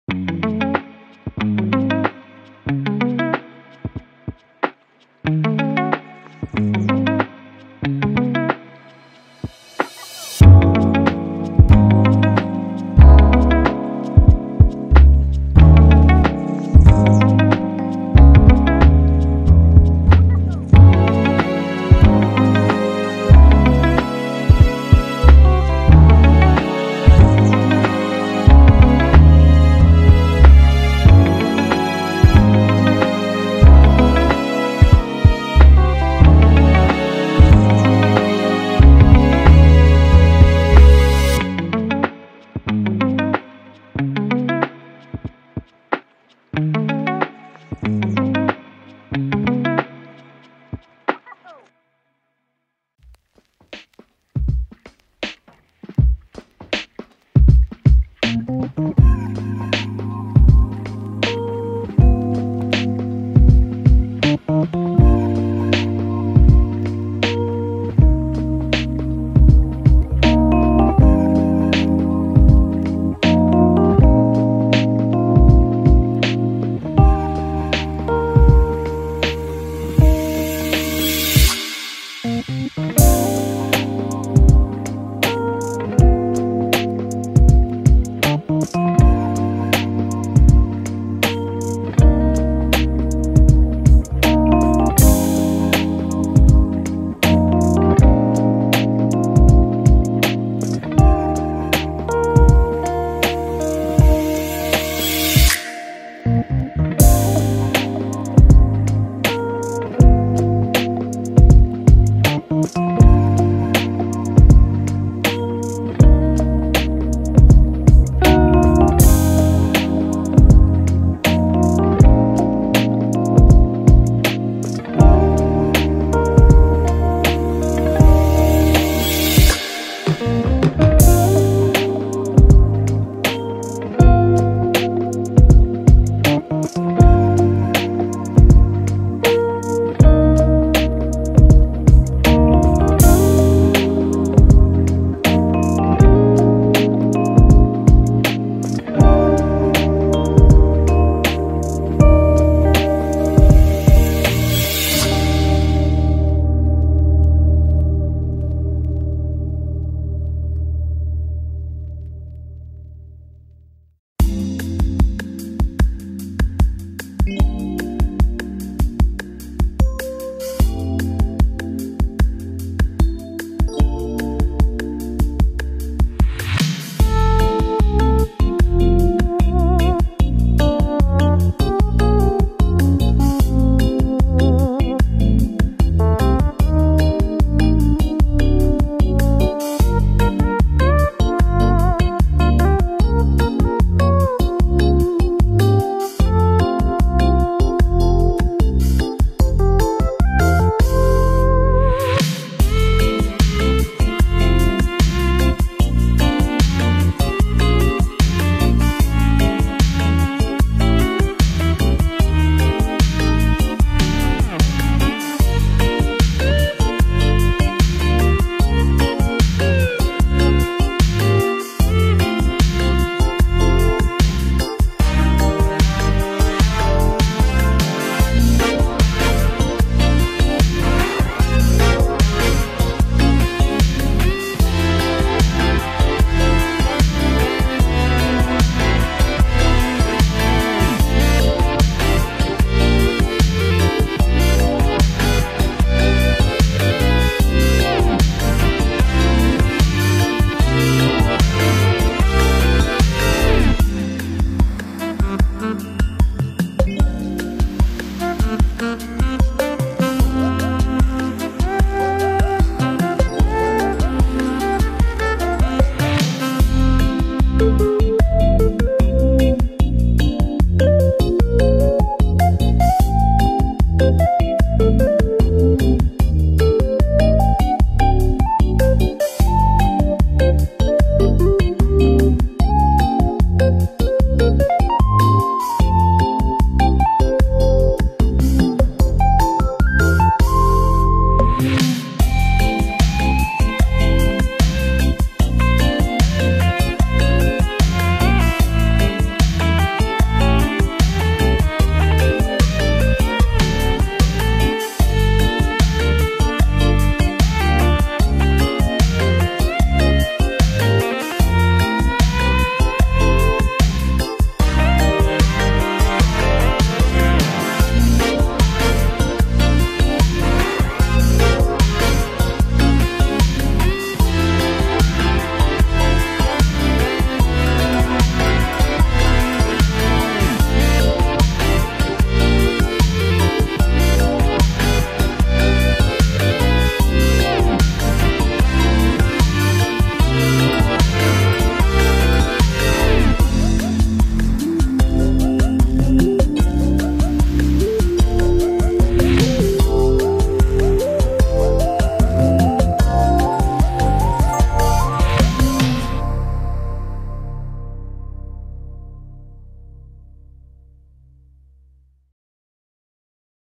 Hawaii 2024 - Start & Waikiki
This is the first chapter of my journey to Hawaii: The start itself & the first two days in Honolulu & Waikiki. I hope you'll enjoy watching it!